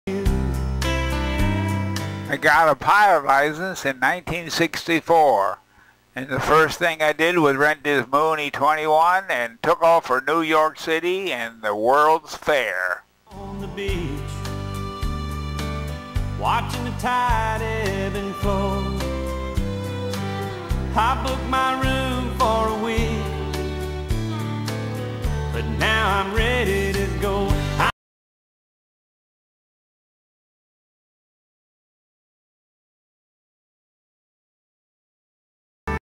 World fair2
1964 got private pilot license, rented this Mooney 21 and flew to NY City with family for the World's Fair.